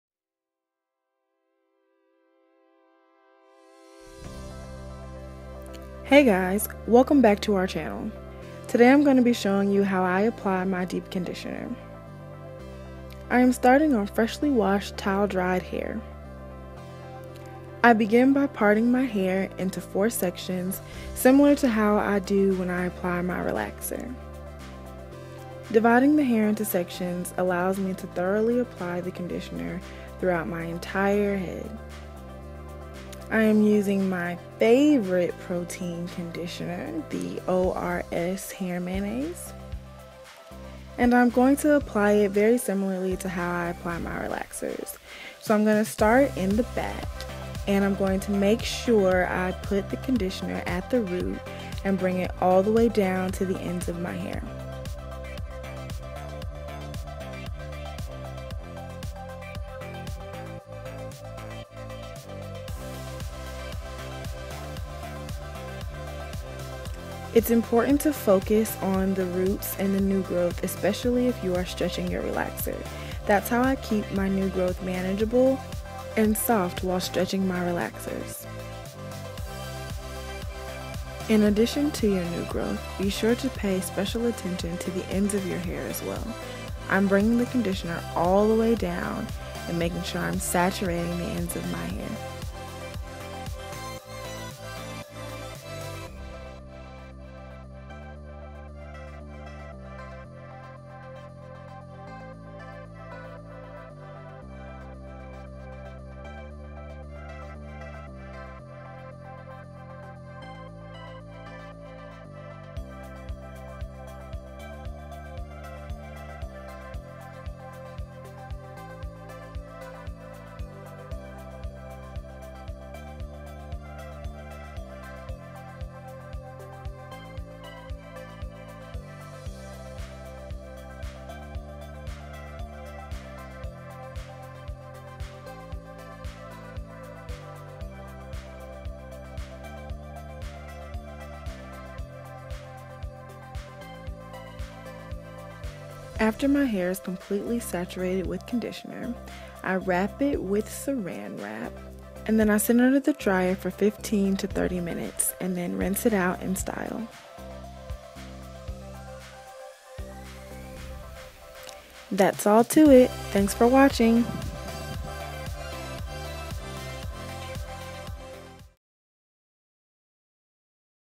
Relaxed Hair Deep Condition Routine (tutorial)
PRODUCT USED:
ORS Hair Mayonnaise